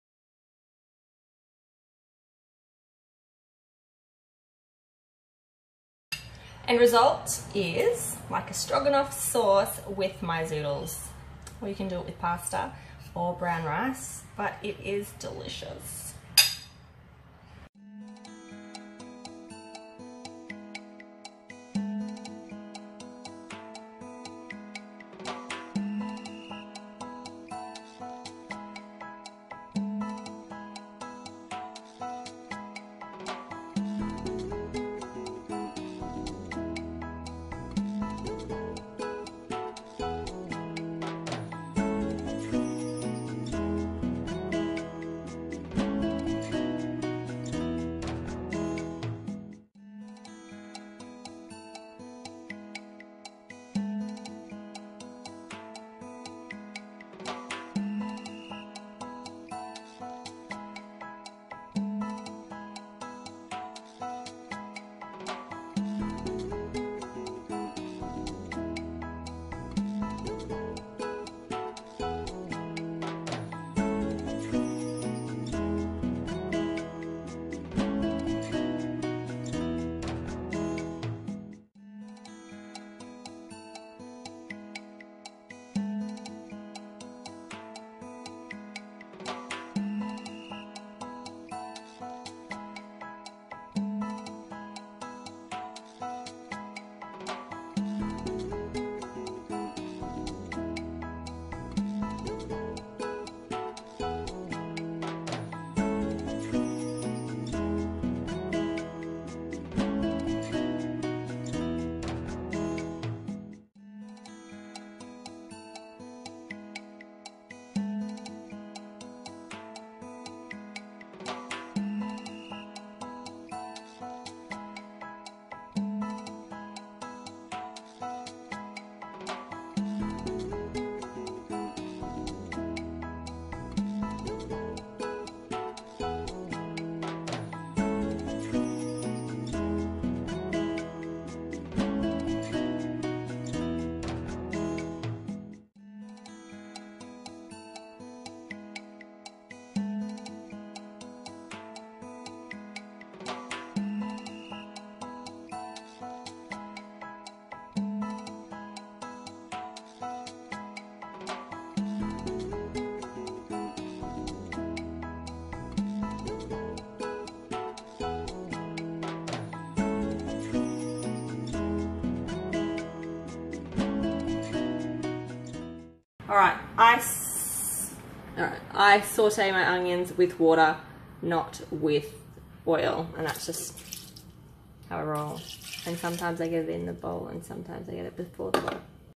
Plant based creamy mushroom sauce
Simple, versatile and yummy vegan sauce. Remember to soak 100g of cashews for a few hours before you start.